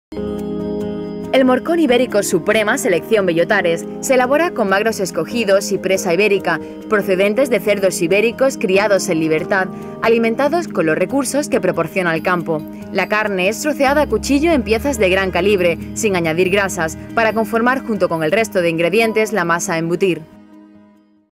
MORCÓN IBÉRICO SELECCIÓN BELLOTARES
Morcón de cerdo ibérico calidad suprema, producido con magros escogidos y presa ibérica, troceados a cuchillo, sin grasas añadidas, curado en tripa y secadero naturales y envasado al vacío tradicional. La caducidad es de un año, desde la fecha de envasado al vacío. A partir de aquí, una vez abierta la bolsa y empezado el consumo, la conservación de la pieza es fácil: en su propia bolsa y en el refrigerador.